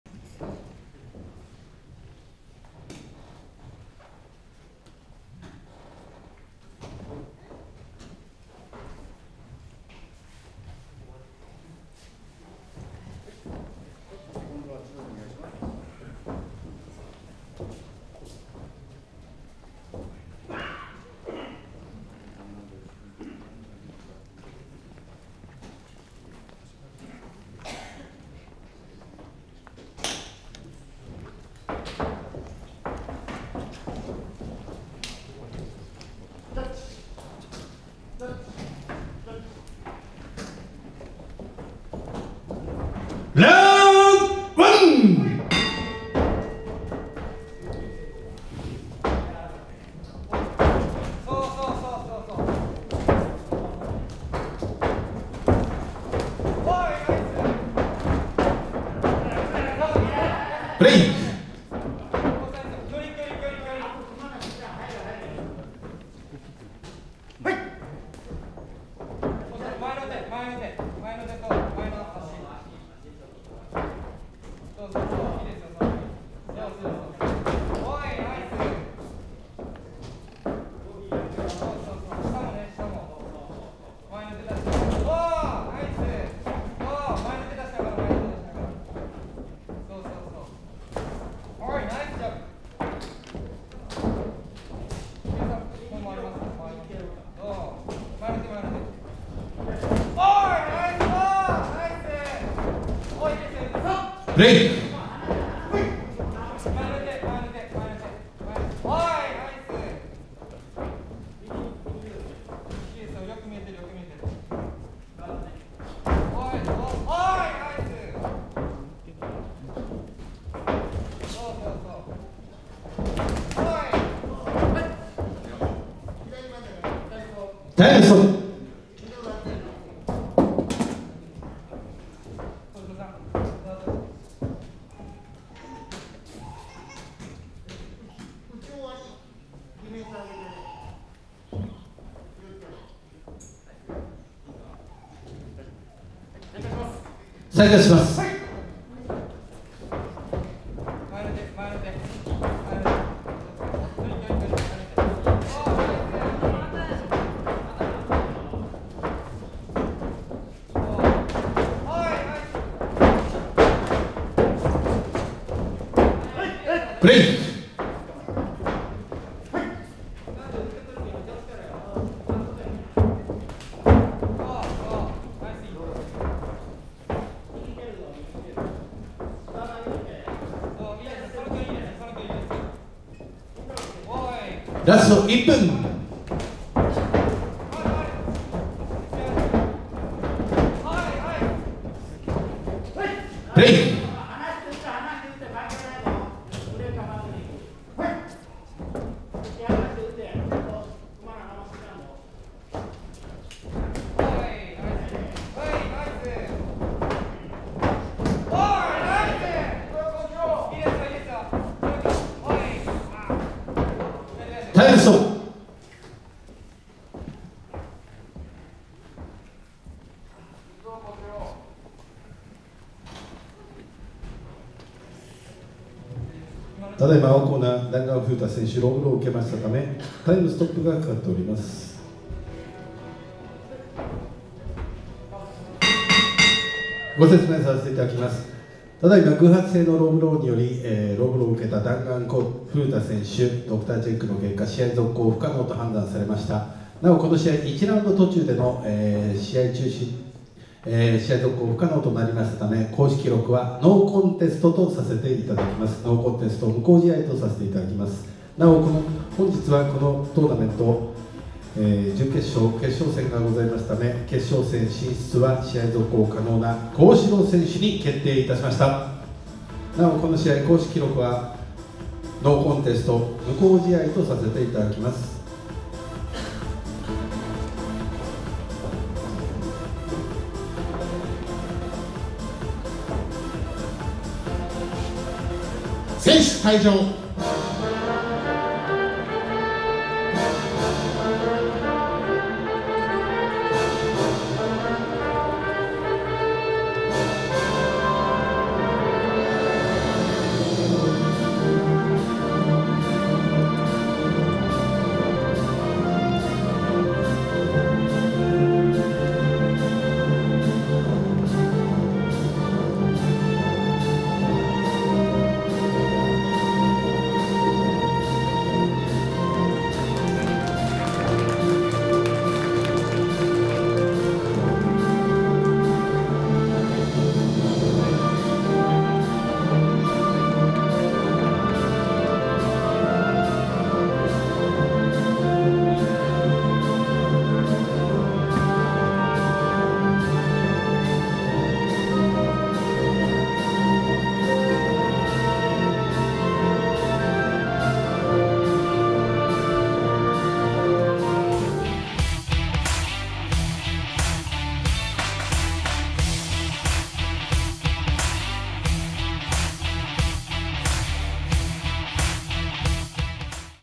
KPKB vol 5 in アクロス福岡【幸四郎 vs 弾丸風太】
《Life Clean Service PRESENTS》

【2022年2月13日 KPKB vol.5 in アクロス福岡】
【KPKB第2代バンタム級王座決定トーナメント準決勝】
【幸四郎 vs 弾丸風太】

2022年2月13日に行われたKPKB vol.5のプロ戦を
YouTubeにて限定公開！熱き闘いを是非ご覧ください。

【▼協賛企業様】